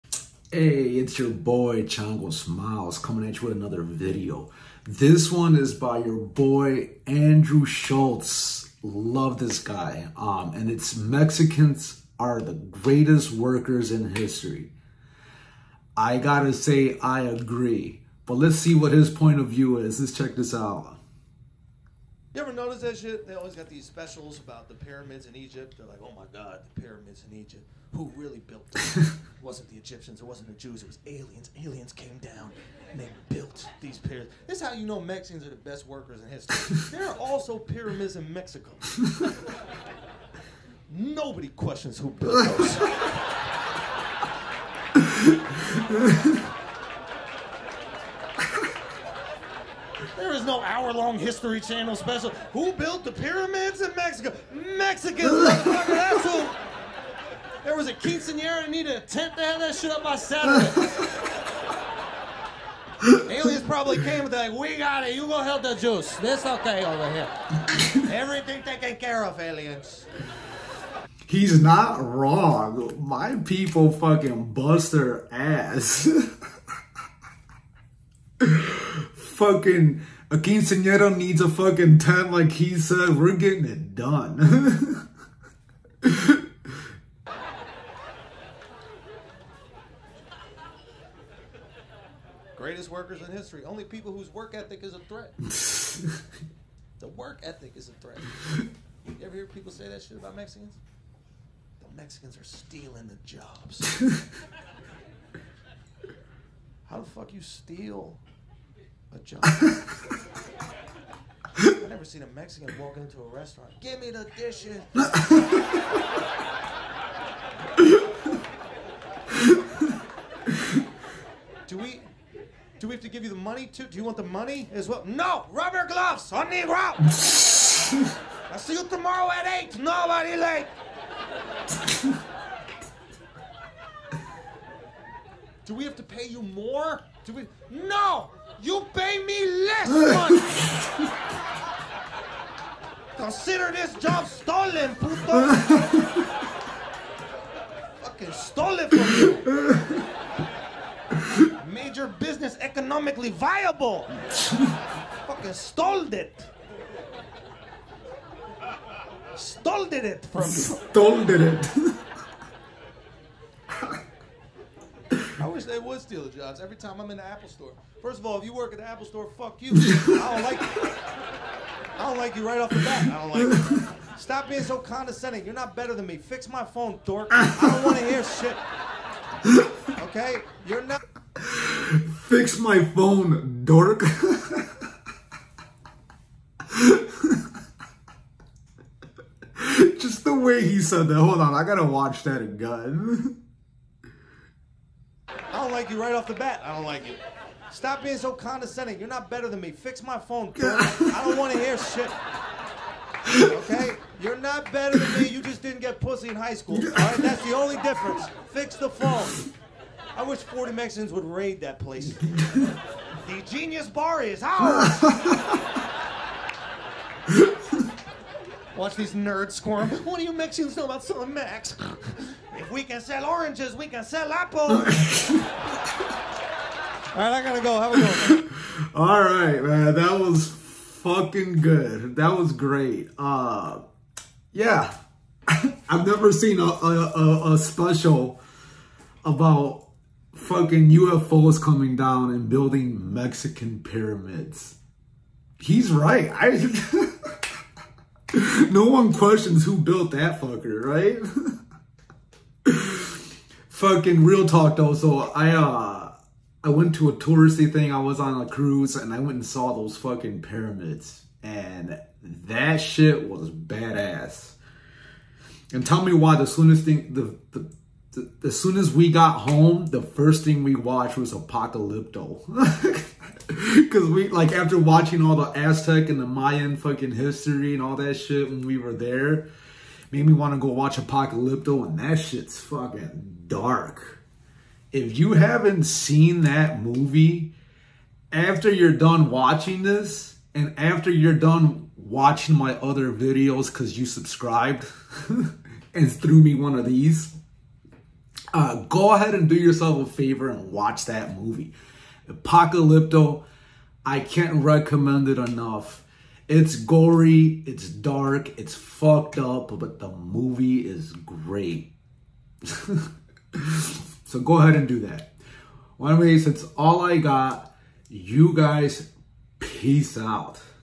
FIRST TIME REACTING TO | MEXICANS are the GREATEST workers in history (EXTENDED CLIP)
FIRST TIME REACTING TO | MEXICANS are the GREATEST workers in history (EXTENDED CLIP) - Andrew Schulz - Stand Up Comedy